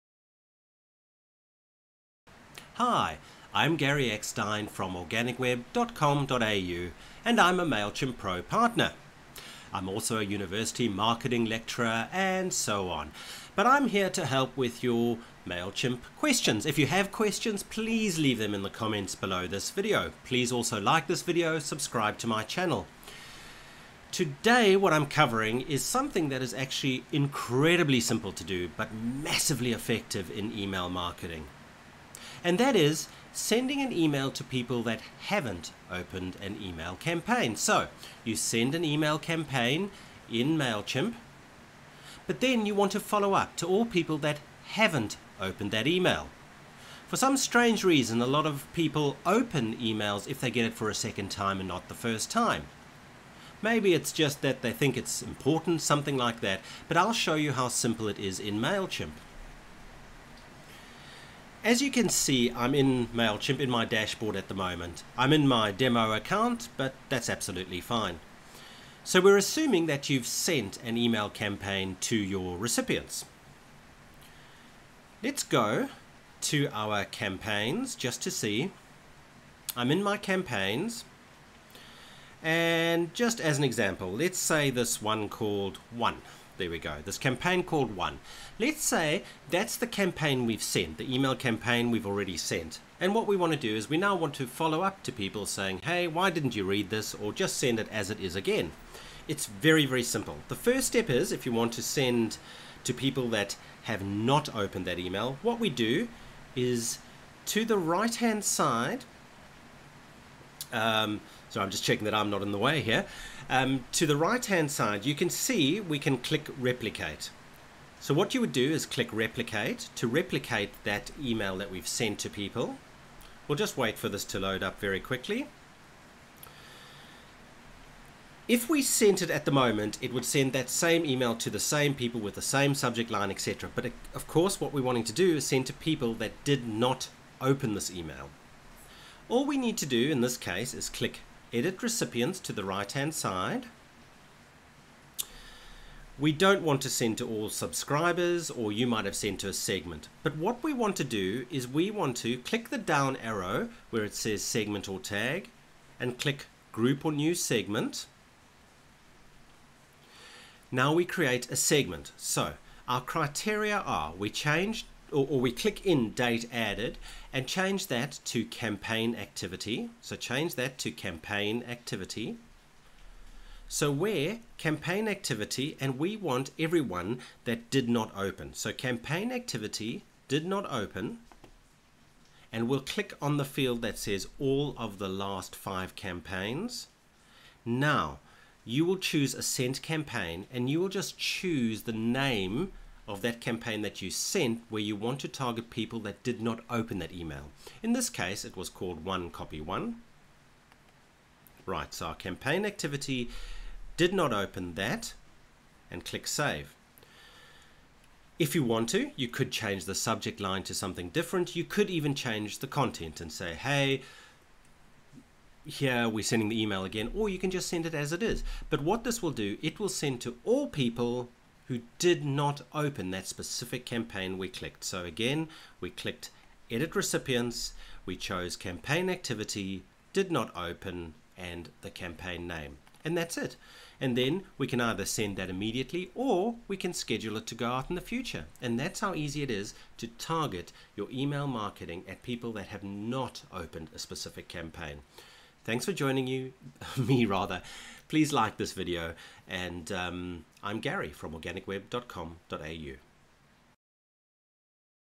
[Mailchimp] How to Send an Email to Subscribers That Didn't Open an Email 💯
Do you want to send an email marketing campaign to all subscribers that didn't open your previous email? This is very simple to do in Mailchimp.

In this short tutorial Gary Eckstein, a Mailchimp Pro Partner, gives instructions on how to select all contacts that were sent a previous email but didn't open the email. Gary illustrates how to resent a previous campaign to non-openers.

This short instructional videos will boost your Mailchimp email marketing.